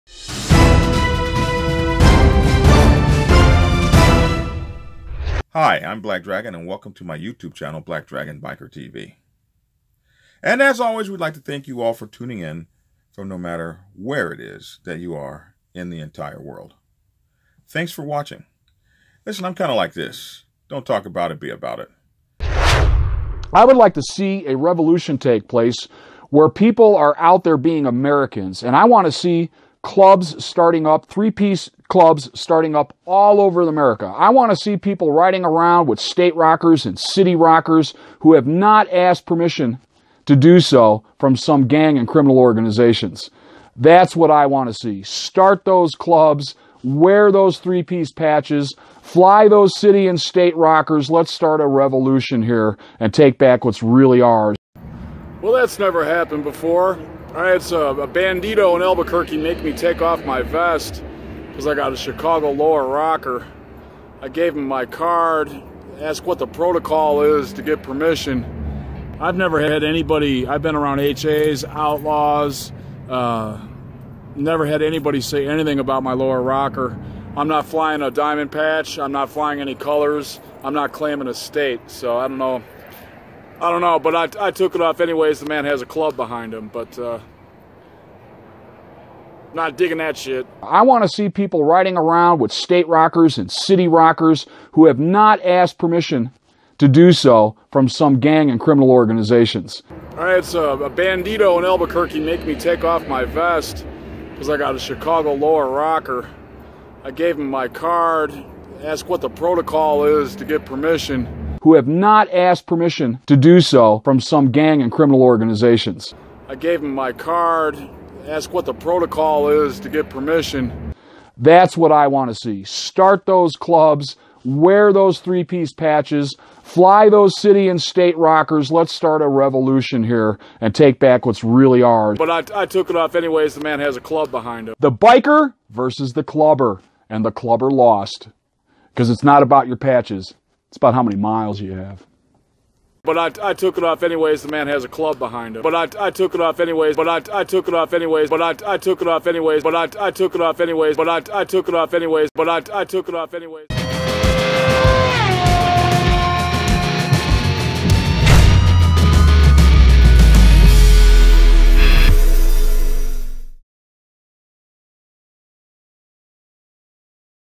When the 1%er Say's "Take That Rocker Off!"/First Amendment Audit Fail
You can tell the youngsters to go start a revolution and risk their lives but when you aren't willing to do it yourself it's cowardly. The story changed quickly when you were forced to confront it face-to-face, when you were told to take your rocker off. And you did exactly what you needed to do to keep yourself out of harm's way. That's because confronting overwhelming opposition is never as easy as it is to talk about it.